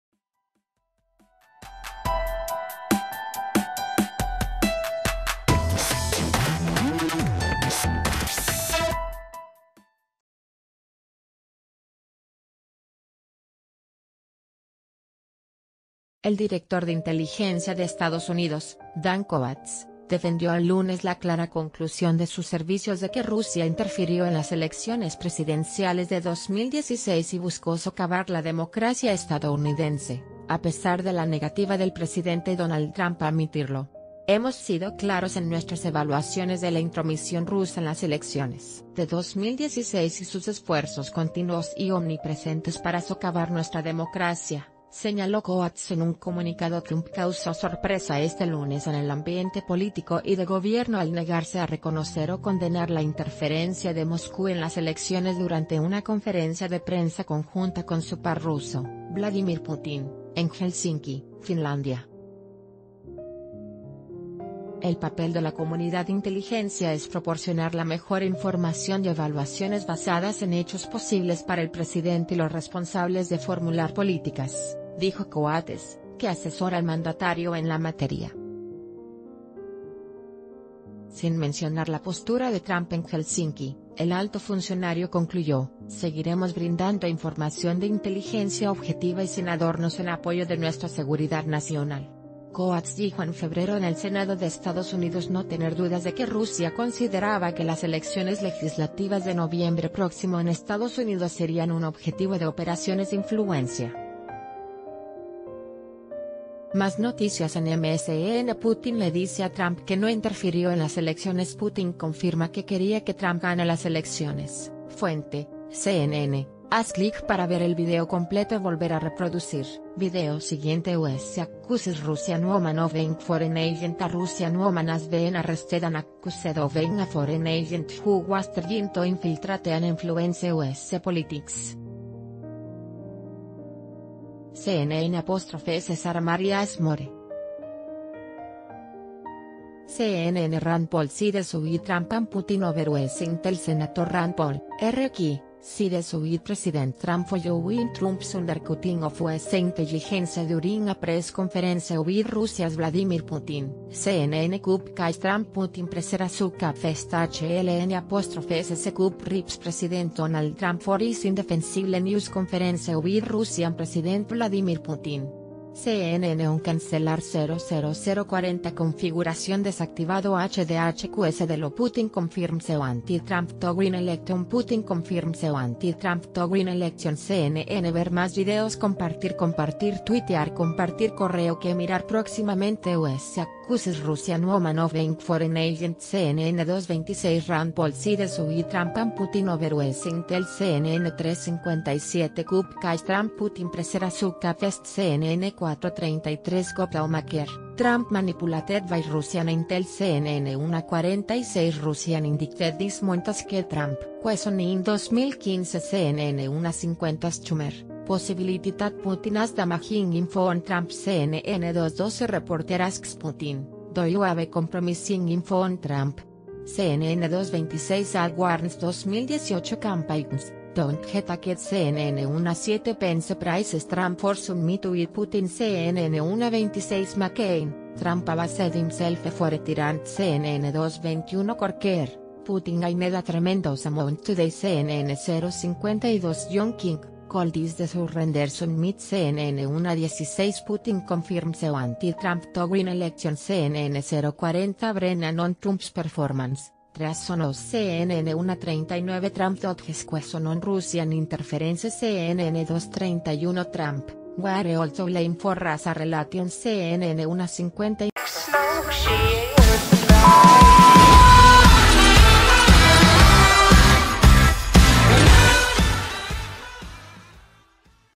Jefe de inteligencia EEUU defiende conclusión de que Rusia interfirió en elecciones
El director de Inteligencia de Estados Unidos, Dan Coats, defendió el lunes la "clara" conclusión de sus servicios de que Rusia interfirió en las elecciones presidenciales de 2016 y buscó "socavar" la democracia estadounidense, a pesar de la negativa del presidente Donald Trump a admitirlo."Hemos si...